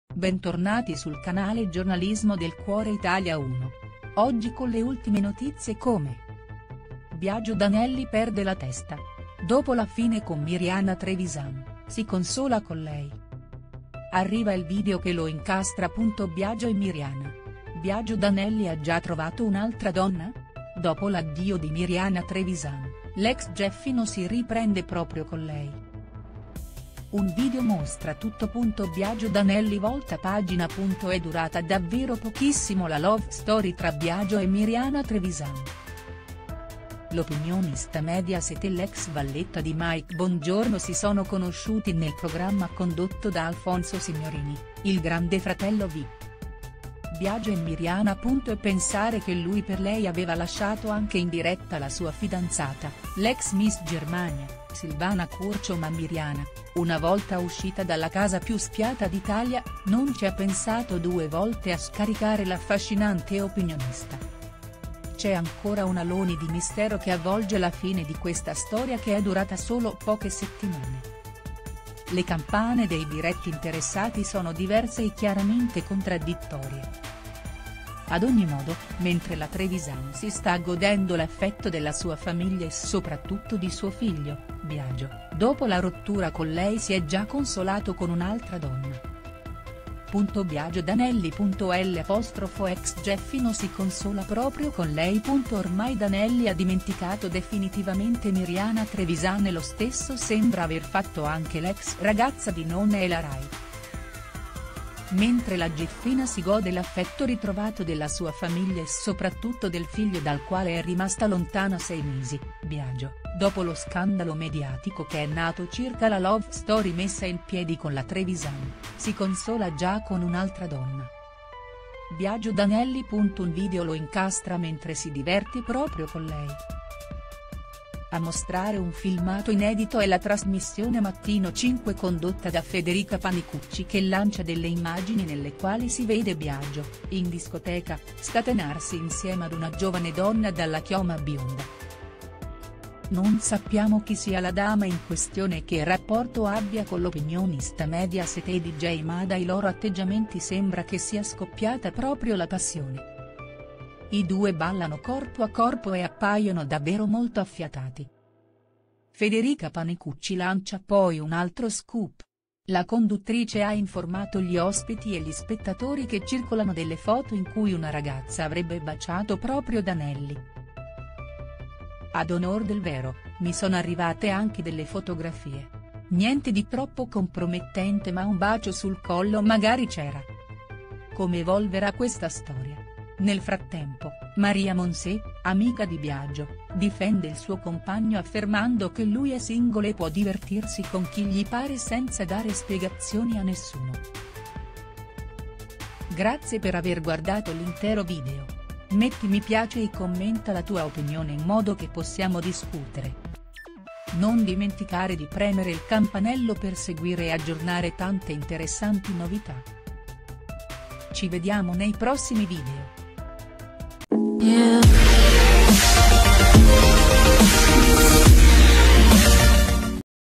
Biagio D’Anelli fuori di sé, dopo Miriana si consola proprio con lei il bacio è proibito
Biagio D’Anelli ha già trovato un’altra donna? Dopo l’addio di Miriana Trevisan, l’ex gieffino si riprende proprio con lei. Un video mostra tutto.

Biagio D’Anelli volta pagina
È durata davvero pochissimo la love story tra Biagio e Miriana Trevisan. L’opinionista Mediaset e l’ex valletta di Mike Bongiorno si sono conosciuti nel programma condotto da Alfonso Signorini, il Grande Fratello Vip.